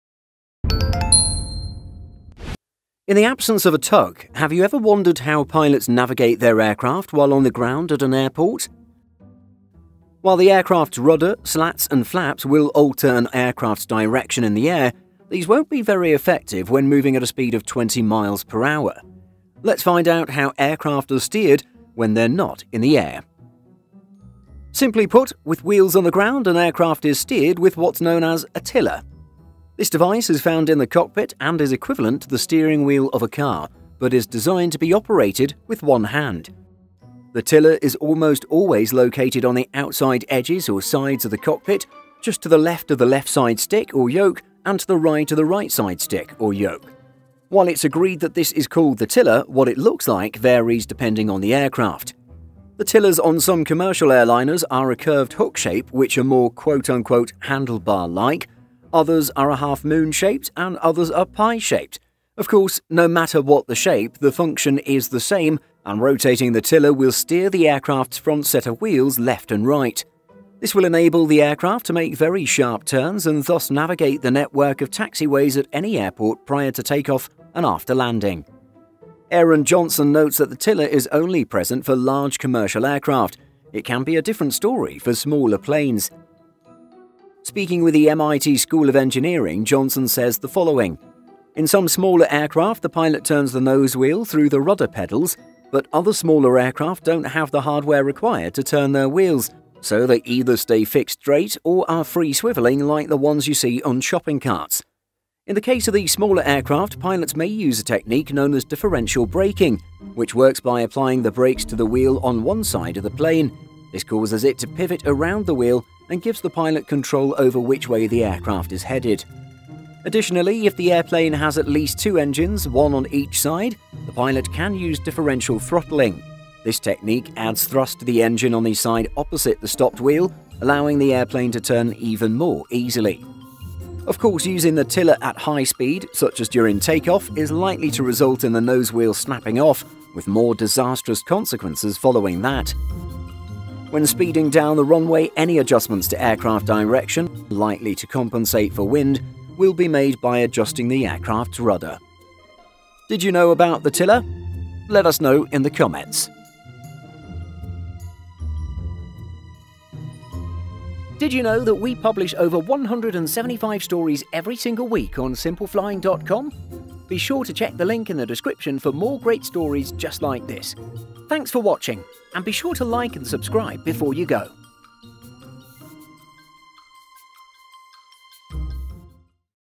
How Do Pilots Steer Passenger Jets When On The Ground?
In the absence of a tug, have you ever wondered how pilots navigate their aircraft while on the ground at an airport?

While the aircraft’s rudder, slats, and flaps will alter an aircraft’s direction in the air, these won’t be very effective when moving at a speed of 20 miles per hour.

Let’s find out how aircraft are steered when they’re not in the air.